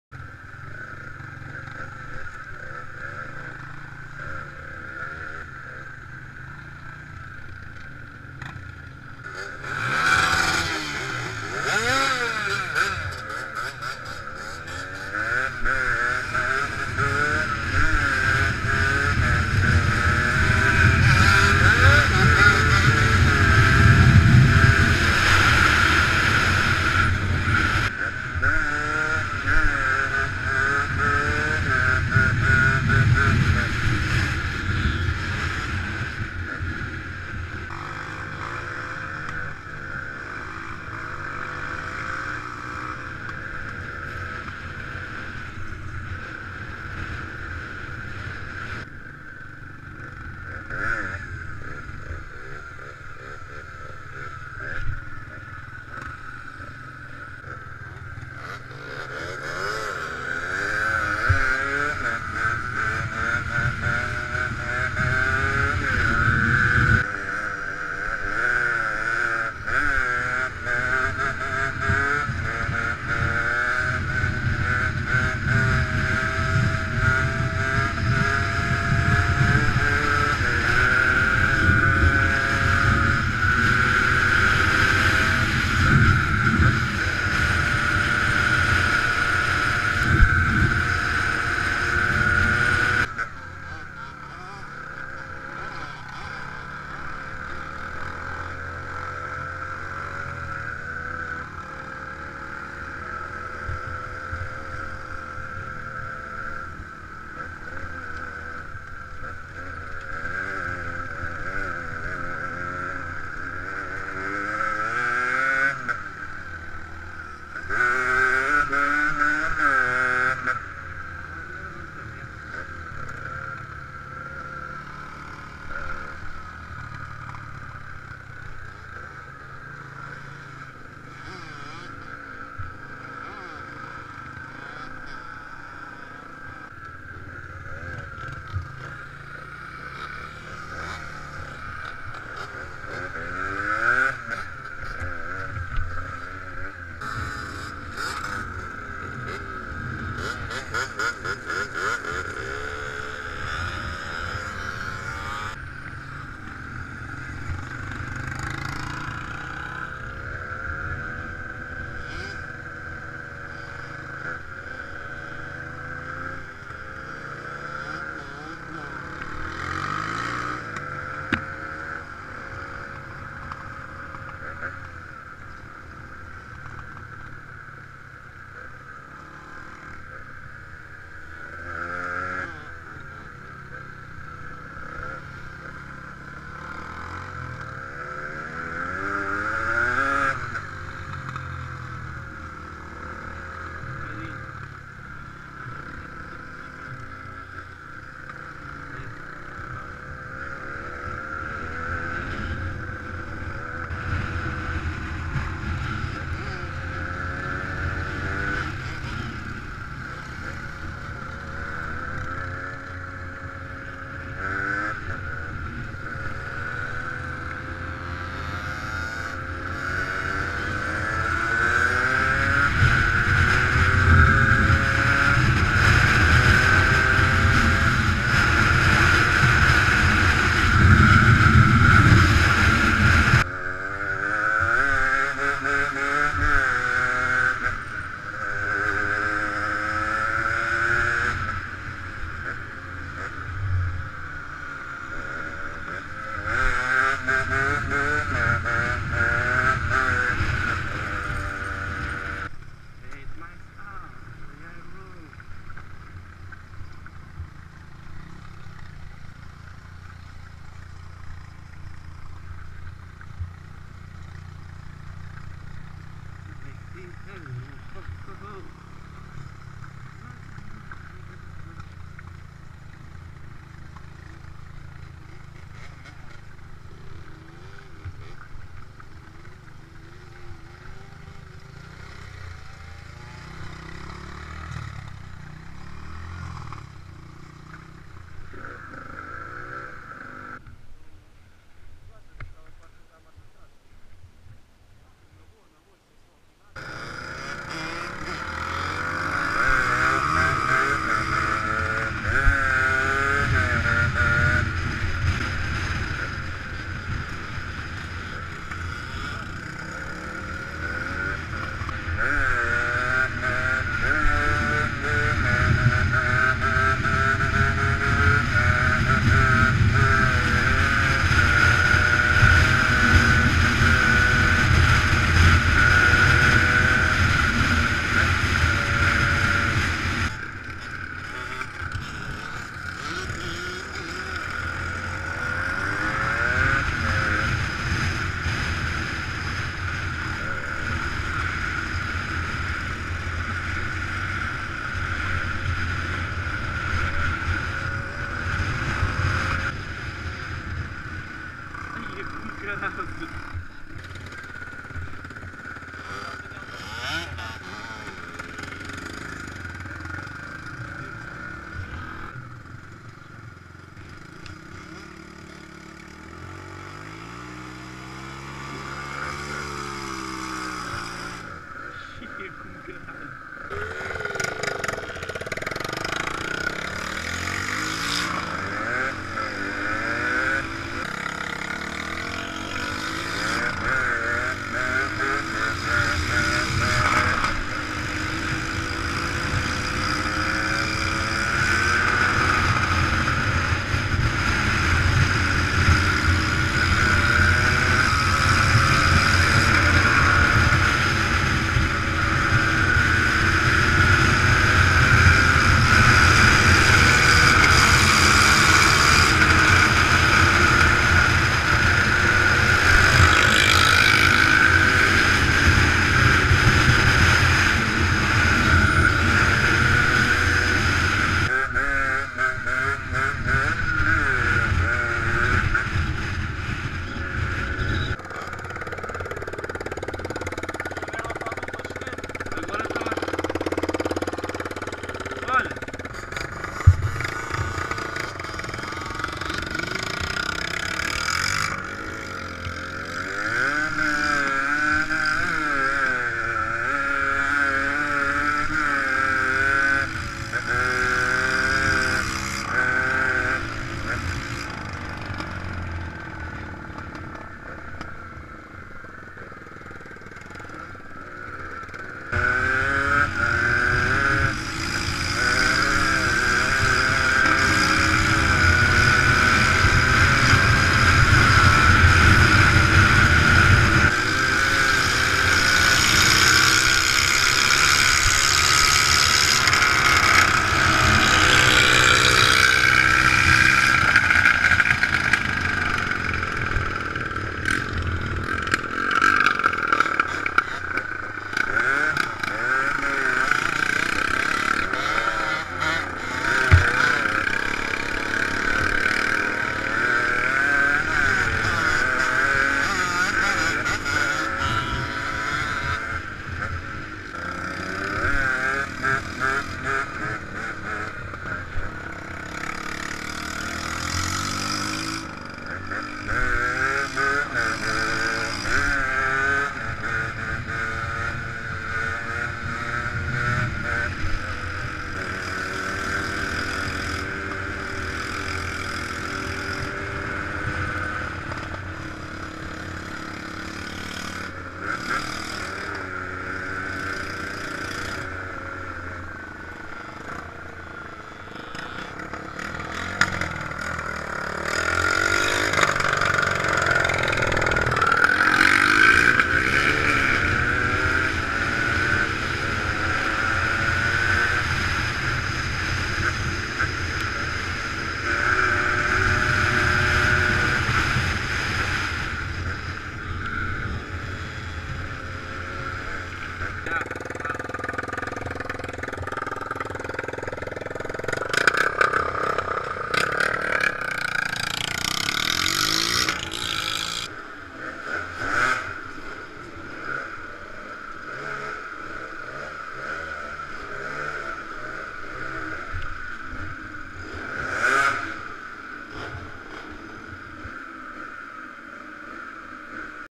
Yamaha DT80, ktm sx65 and Yamaha warrior 350 ripping in the woods
Just having fun on the bikes, poping some wheelies, and ripping in those woods.